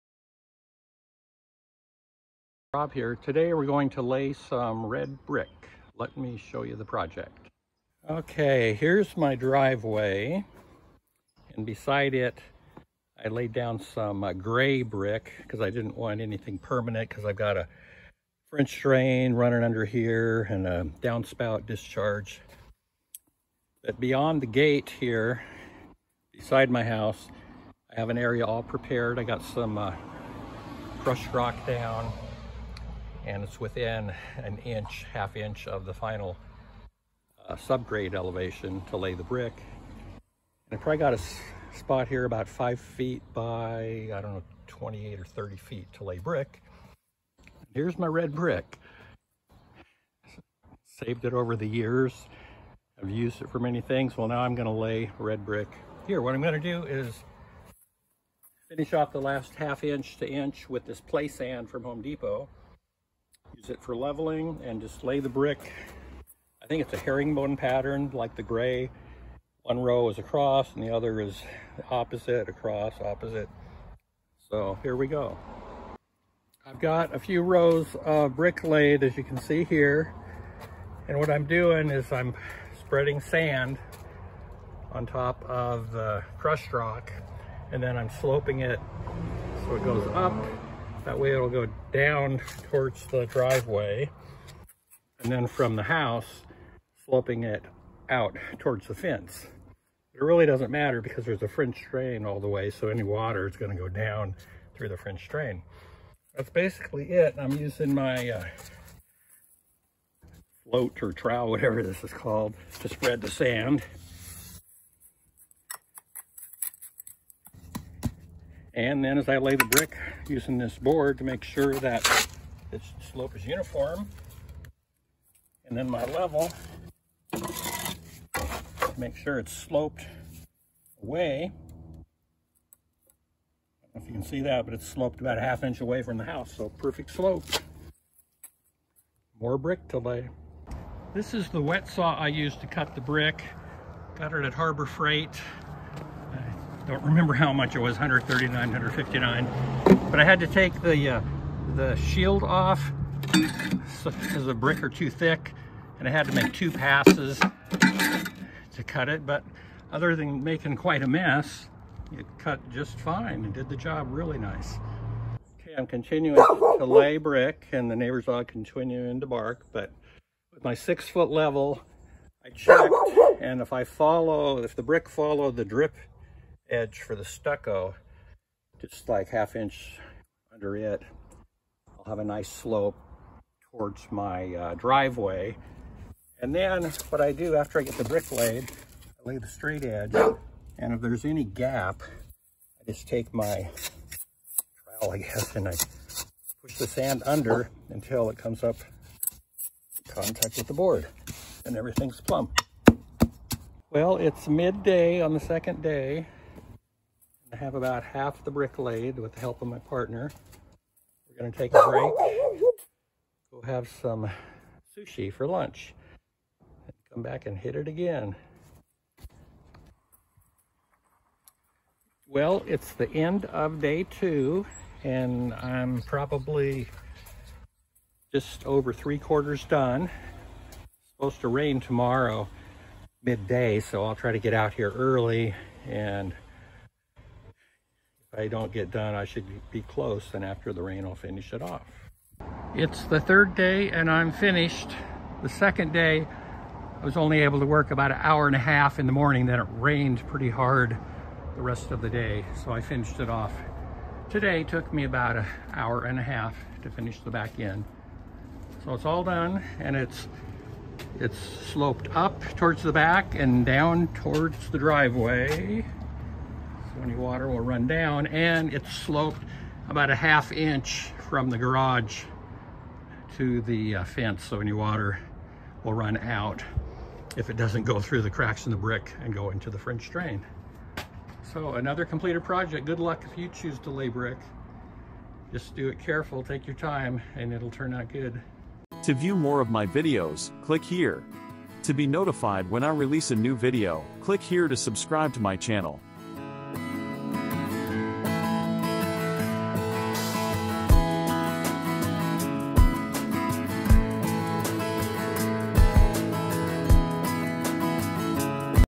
Construct a Brick Walkway
Transform your outdoor space with this simple and elegant DIY red brick walkway! In this video, I’ll show you how to construct a small brick path along the side of my garage, perfect for adding functionality and charm to your home.

You’ll learn:
✅ How to prepare the area and measure for your walkway
✅ Tips for leveling the ground and creating a sturdy base
✅ Step-by-step techniques for laying red bricks evenly
✅ Finishing touches to ensure durability and a polished look

Whether you're a seasoned DIYer or a beginner, this project is both manageable and rewarding.

Tools and Materials:

Red bricks
Sand and gravel
Level
Shovel
Rubber mallet
Measuring tape

Let’s get building! 🚧